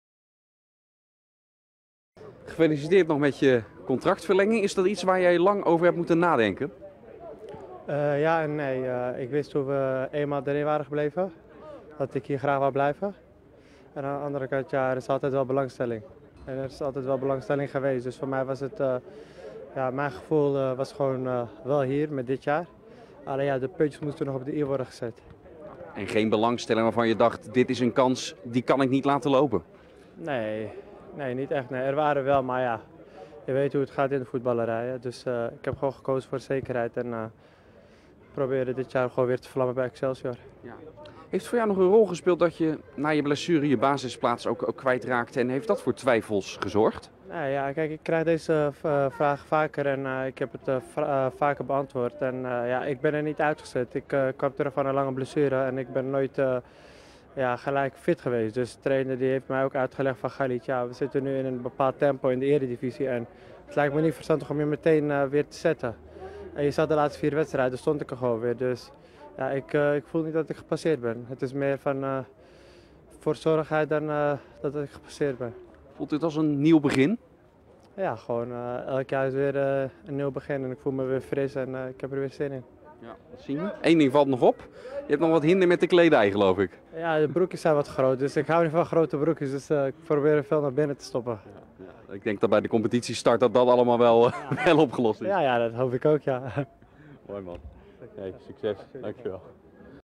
Khalid Karami over zijn contractverlenging en blessure
Excelsior is begonnen aan de trainingen voor het nieuwe seizoenen, maar klaar voor de seizoensstart zijn ze op Woudestein nog niet. Directeur Ferry de Haan laat weten dat hij voor elke linie op zoek is naar een directe versterking.

   Lees meer: Excelsior aast op versterking in elke linie